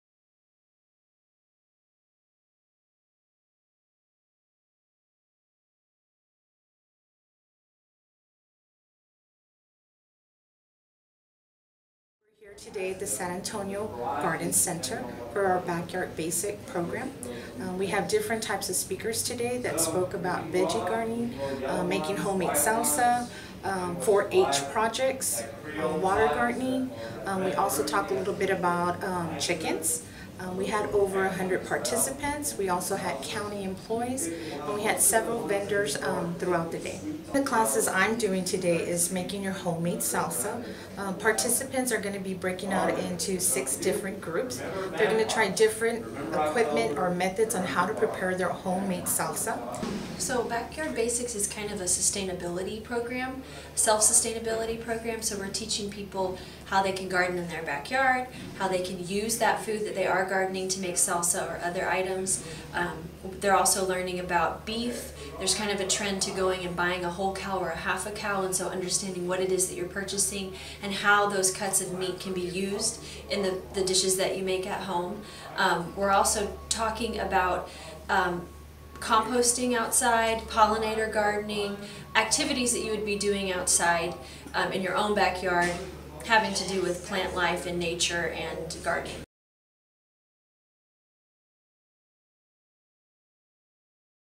Backyard Basics 2018
The 2018 Backyard Basics at the San Antonio Garden Center program introduced attendees to a variety of ways to be more self-sustaining through growing, preserving  and preparing their own food.